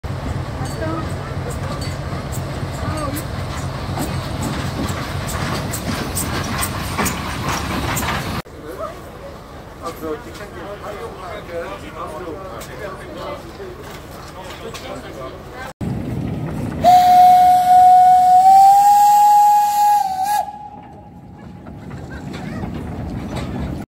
Les vieux trains miniers ont roulé
La saison a repris ce week-end pour les trajets en train à travers les anciens sites miniers, entre Fond-de-Gras et Pétange.